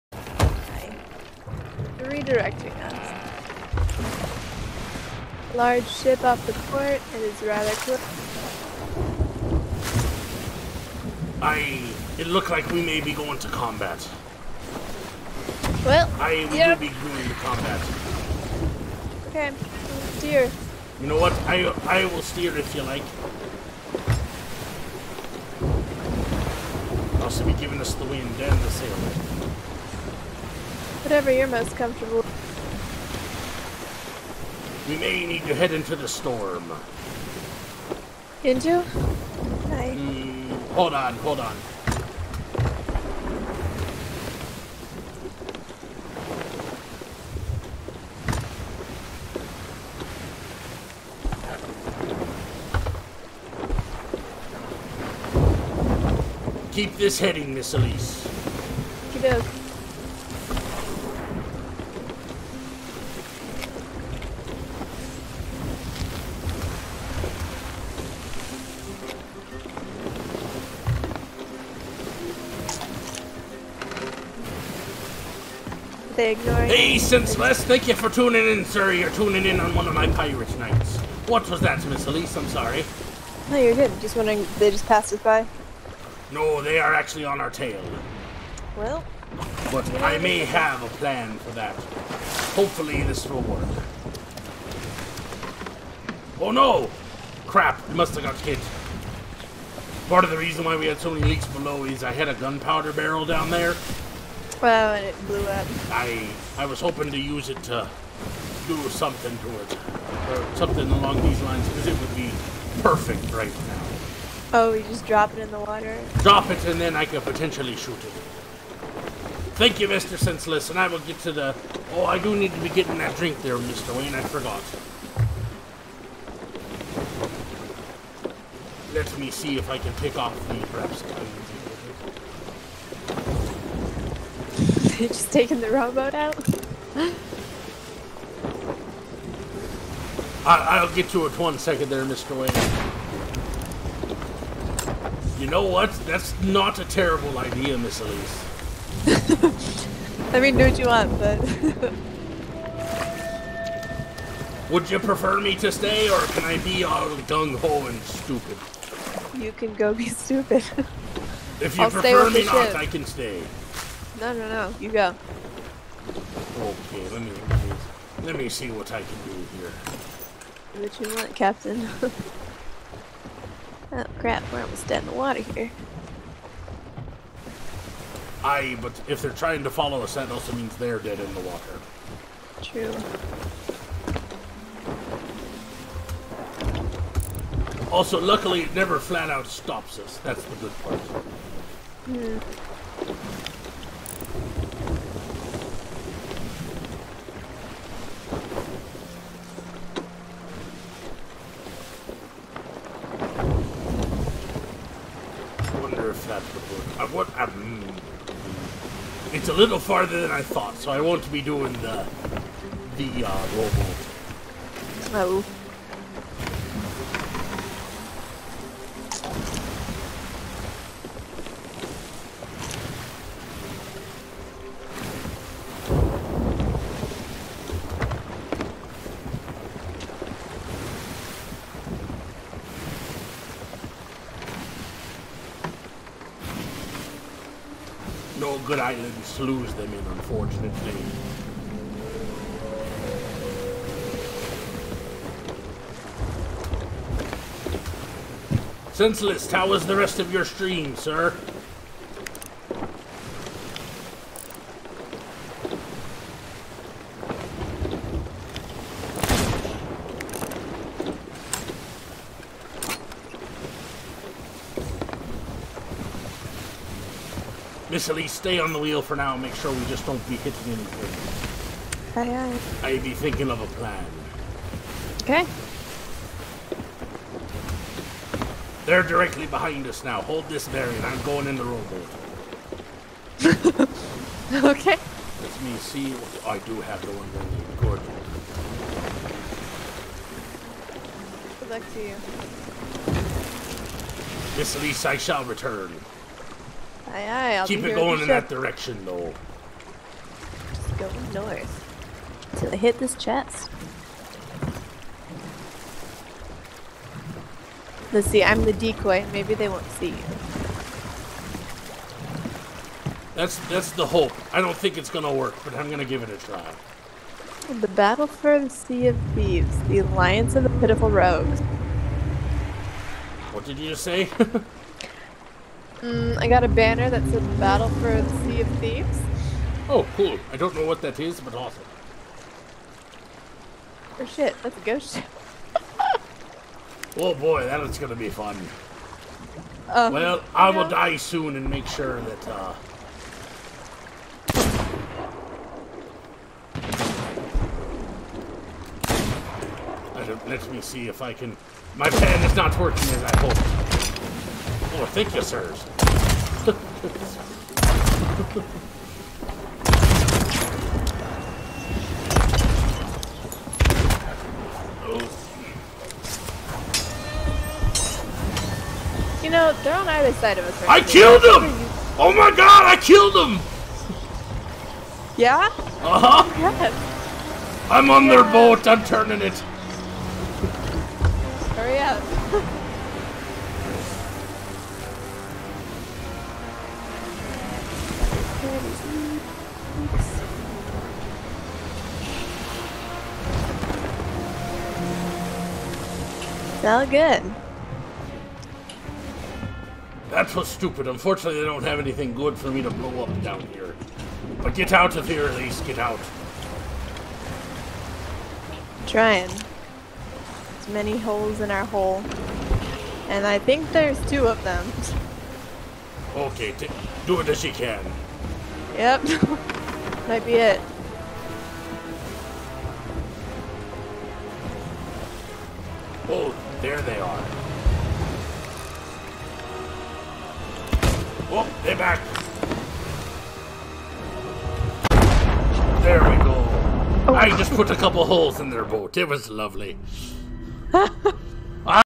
The Brig never knew what hit them!
With First Mate Elise piloting the boat, Captain Blondebeard was able to get a brig off his tail!